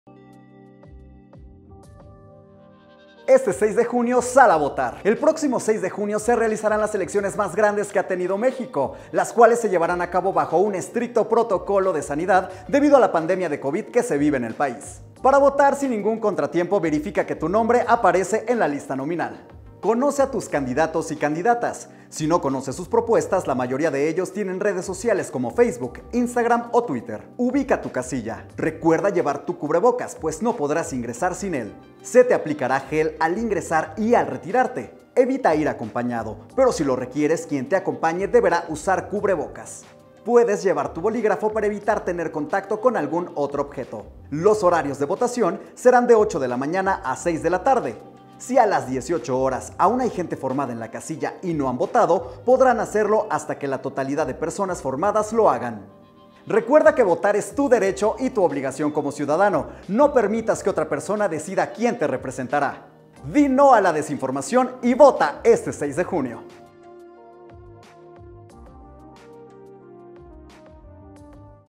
¡ESTES 6 DE JUNIO SAL A VOTAR!Recuerda que votar es tu derecho y tu obligación como ciudadano
¡ESTES 6 DE JUNIO SAL A VOTAR!

Recuerda que votar es tu derecho y tu obligación como ciudadano. No dejes que otras personas decidan quién te representará.